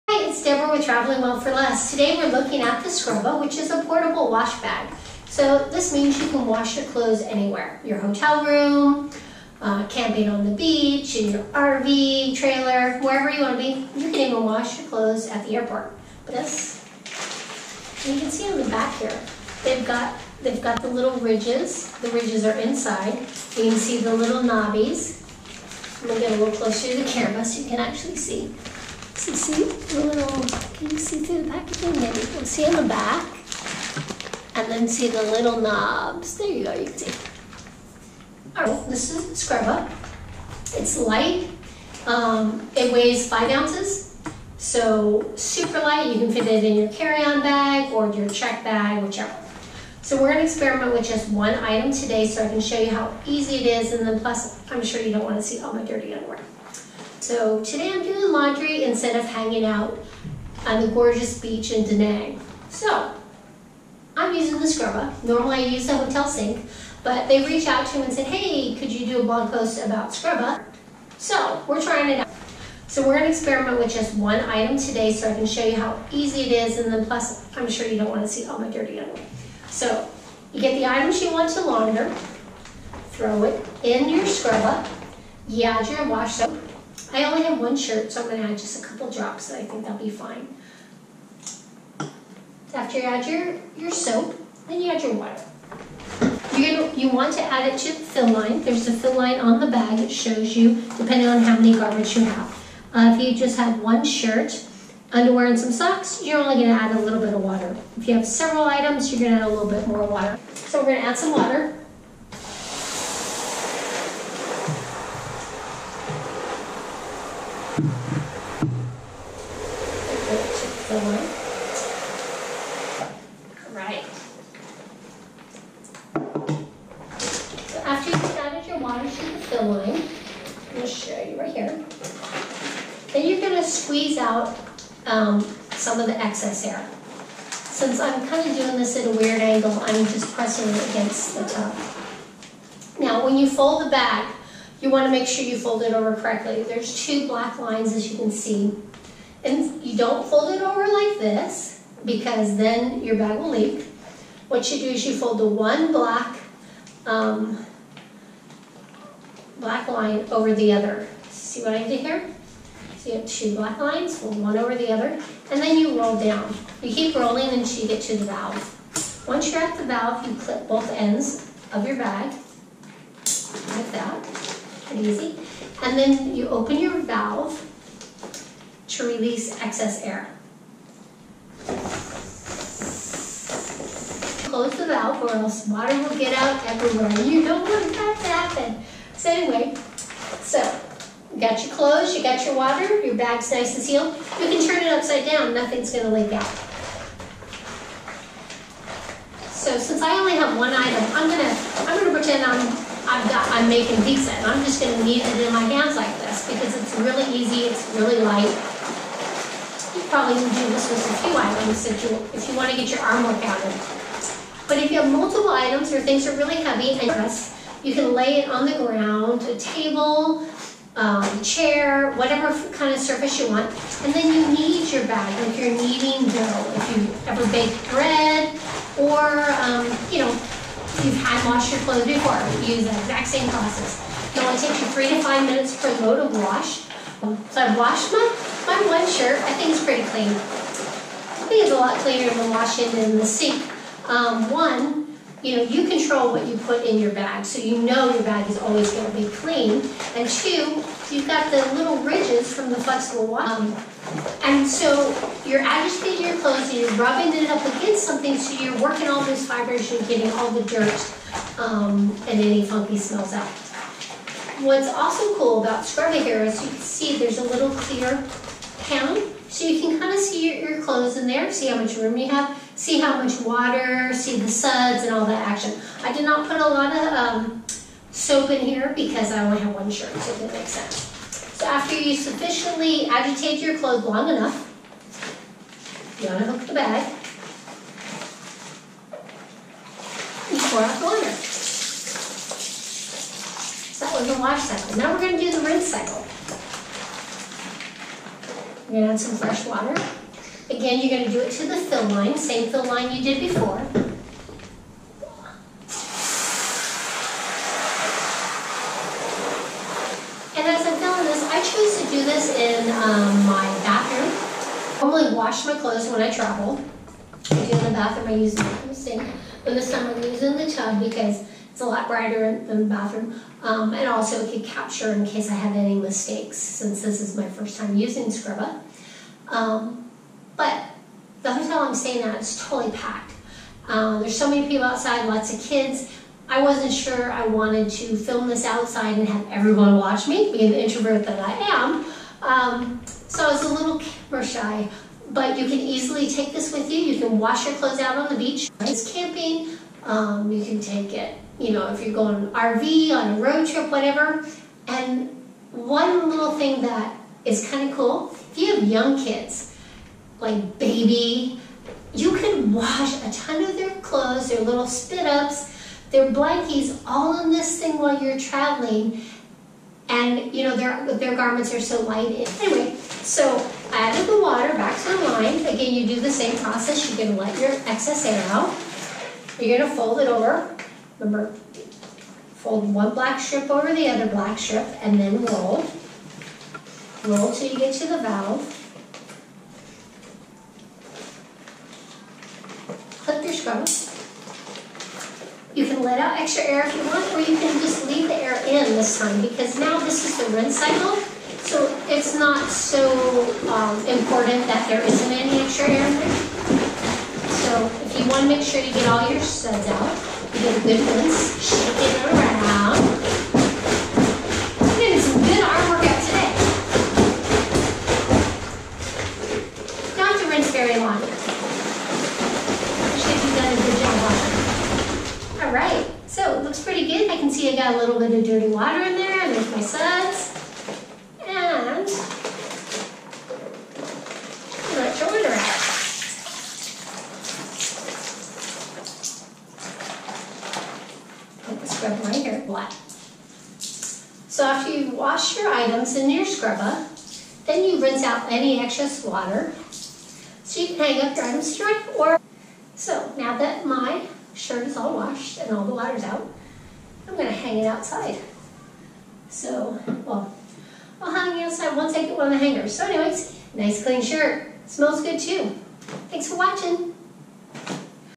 Scrubba Wash Bag Portable Washing Machine Review: EASILY Wash Clothes While Traveling.
Learn how to use a Scrubba Wash Bag Portable Washing Machine. I show you how easy it is to wash your clothes while traveling, camping, hiking with the Scrubba Wash Bag. Have clean clothes anywhere!

This compact, light, eco-friendly laundry bag lets you travel light, avoid excess baggage fees, perfect for vanlife, and keeps your clothes clean. It should be part of your travel gear essentials. Whether you're a solo traveler, on a couples trip, or vacationing with the family, sometimes you have to wash something.

I love how easy the Scrubba is to use, it fits in my carryon bag, and my clothes come out cleaner than washing in the hotel sink.

Advertiser Disclosure: This was a sponsored video on behalf of Scrubba. All opinions are 100% mine.